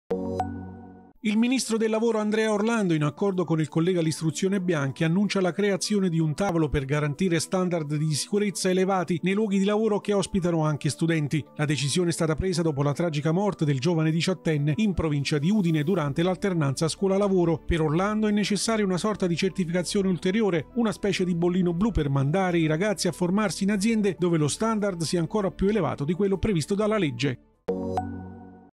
Scuola-lavoro, Orlando: "Ci vuole bollino blu"
"Necessario una sorta di bollino blu per mandare i ragazzi a formarsi in aziende, dove lo standard" di sicurezza "sia ancora più elevato di quello previsto dalla legge". Così il ministro del lavoro Andrea Orlando, che annuncia un tavolo con il collega dell'Istruzione Patrizio Bianchi per garantire maggiore sicurezza all'alternanza scuola-lavoro, dopo la morte di uno studente in un cantiere.